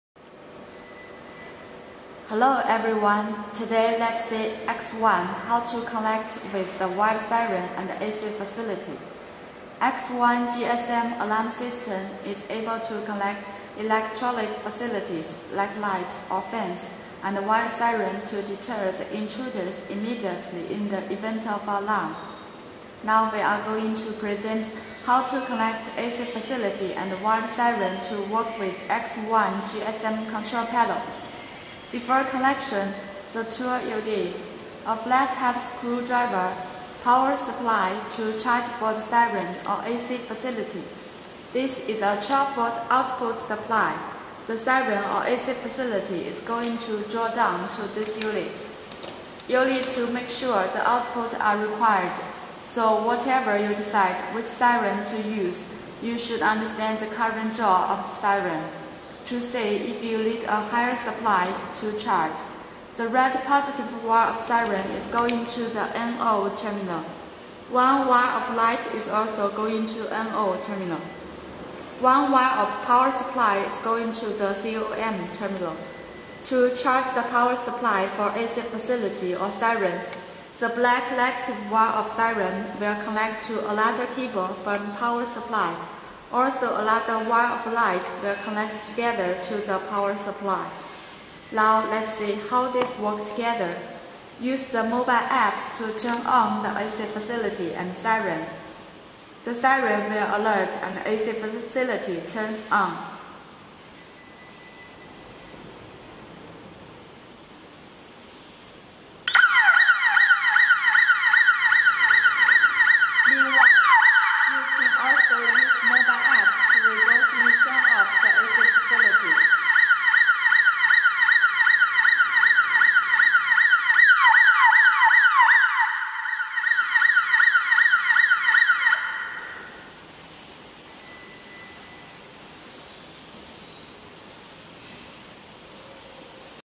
Connect Electrical Facility and Wired Siren
Maxkin X1 GSM alarm system can connect with electrical facilities, like lights, electrical fans...when the sensors are triggered, the facility also will be triggered automatically to deter the intruders. You can also use the APP electrical facility button to turn on/off the function.